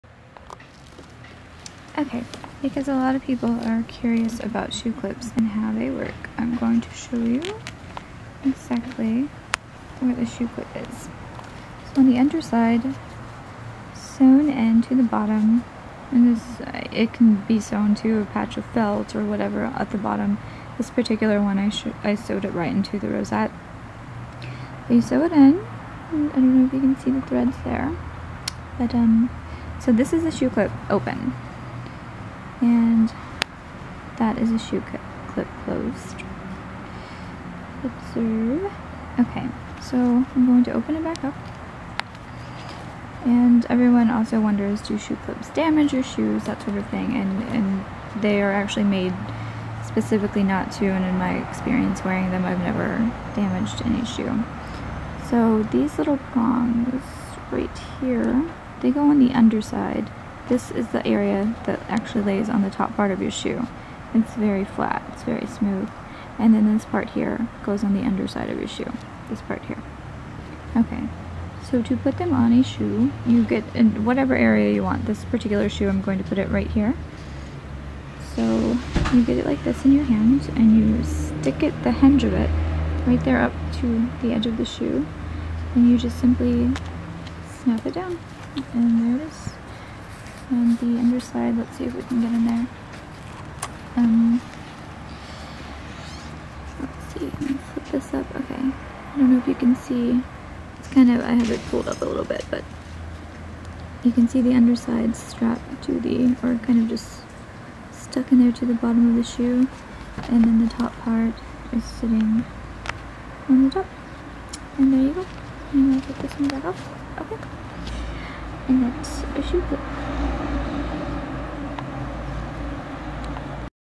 Shoe Clip Demonstration
Soooo...I've gotten a lot of questions about the shoe clips listed in my Etsy shop respectyourmother.etsy.com so I made a quick little video.

This isn't a "how to" video on making shoe clips rather it answers the questions "what is it?" or "how does is work?" and "does it damage my shoe?"

I hope this helps, let me know if anyone has any additonal questions.